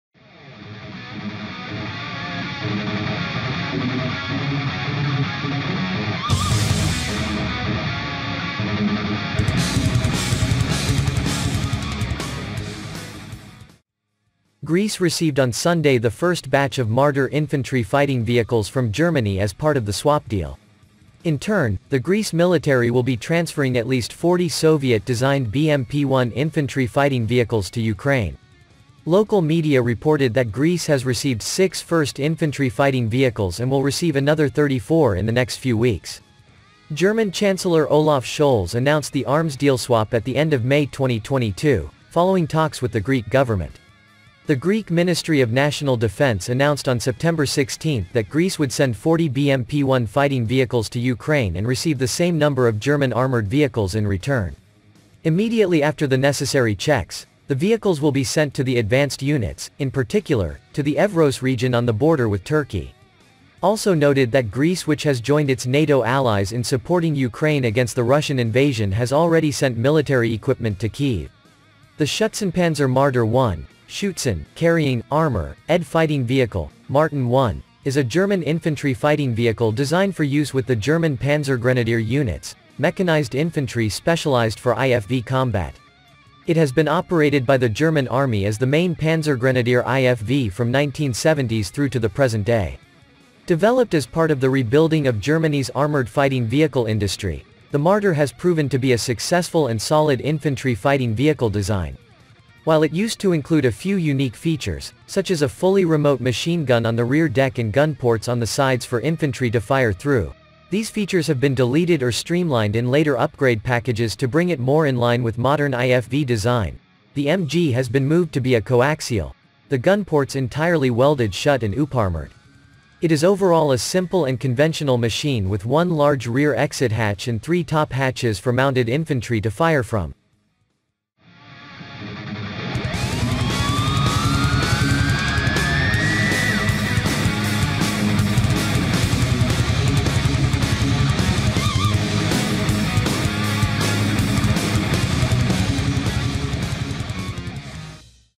Greece receives first batch of Marder fighting vehicles from Germany
Greece received on Sunday the first batch of Marder infantry fighting vehicles from Germany as part of the swap deal.

In turn, the Greece military will be transferring at least 40 Soviet-designed BMP-1 infantry fighting vehicles to Ukraine.

Local media reported that Greece has received six first infantry fighting vehicles and will receive another 34 in the next few weeks.

German Chancellor Olaf Scholz announced the arms deal swap at the end of May 2022, following talks with the Greek government.

The Greek Ministry of National Defense announced on September 16 that Greece would send 40 BMP-1 fighting vehicles to Ukraine and receive the same number of German armored vehicles in return. Immediately after the necessary checks, the vehicles will be sent to the advanced units, in particular, to the Evros region on the border with Turkey.

Also noted that Greece which has joined its NATO allies in supporting Ukraine against the Russian invasion has already sent military equipment to Kyiv.

The Schützenpanzer Marder 1 ("Schützen" carrying "Armour"-ed fighting vehicle "Marten 1") is a German infantry fighting vehicle designed for use with the German Panzergrenadiere units (mechanized infantry specialized for IFV combat). It has been operated by the German Army as the main Panzergrenadiere IFV from 1970s through to the present day. Developed as part of the rebuilding of Germany's armoured fighting vehicle industry, the Marder has proven to be a successful and solid infantry fighting vehicle design. While it used to include a few unique features, such as a fully remote machine gun on the rear deck and gun ports on the sides for infantry to fire through, these features have been deleted or streamlined in later upgrade packages to bring it more in line with modern IFV design (the MG has been moved to be a coaxial, the gun ports entirely welded shut and uparmored). It is overall a simple and conventional machine with one large rear exit hatch and three top hatches for mounted infantry to fire from.